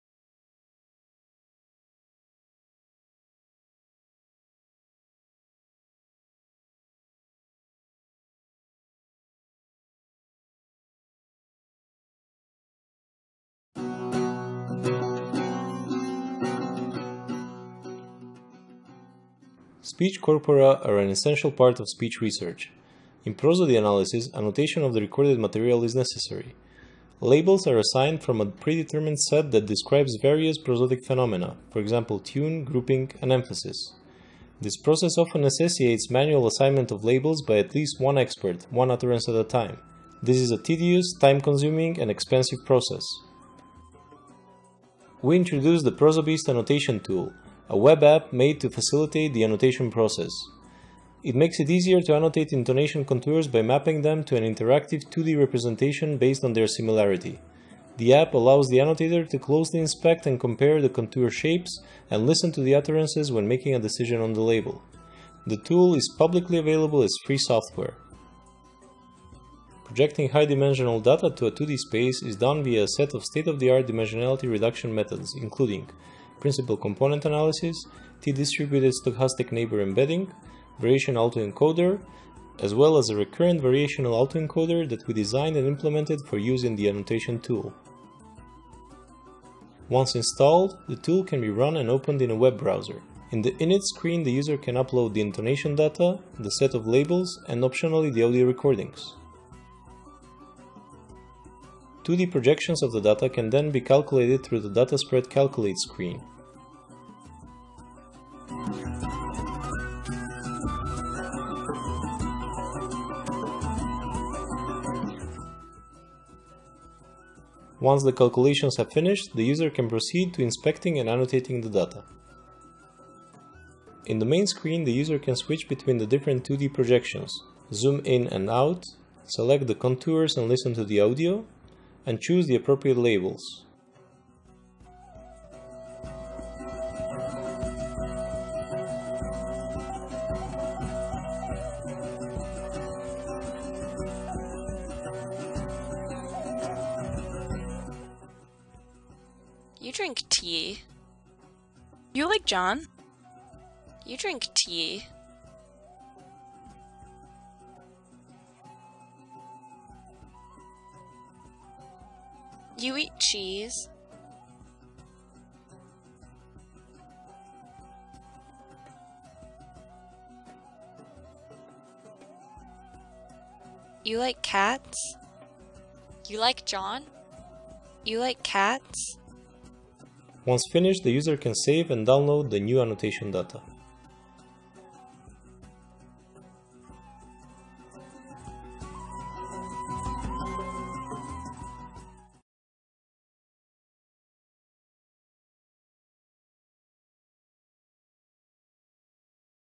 ProsoBeast Prosody Annotation Tool - (3 minutes introduction)
Title: ProsoBeast Prosody Annotation Tool - (3 minutes introduction)

Authors: Branislav Gerazov (UKiM, Macedonia), Michael Wagner (McGill University, Canada)

Category: Prosodic features and structure

Abstract: The labelling of speech corpora is a laborious and time-consuming process. The ProsoBeast Annotation Tool seeks to ease and accelerate this process by providing an interactive 2D representation of the prosodic landscape of the data, in which contours are distributed based on their similarity. This interactive map allows the user to inspect and label the utterances. The tool integrates several state-of-the-art methods for dimensionality reduction and feature embedding, including variational autoencoders. The user can use these to find a good representation for their data. In addition, as most of these methods are stochastic, each can be used to generate an unlimited number of different prosodic maps. The web app then allows the user to seamlessly switch between these alternative representations in the annotation process. Experiments with a sample prosodically rich dataset have shown that the tool manages to find good representations of varied data and is helpful both for annotation and label correction. The tool is released as free software for use by the community.

d02s29t04trim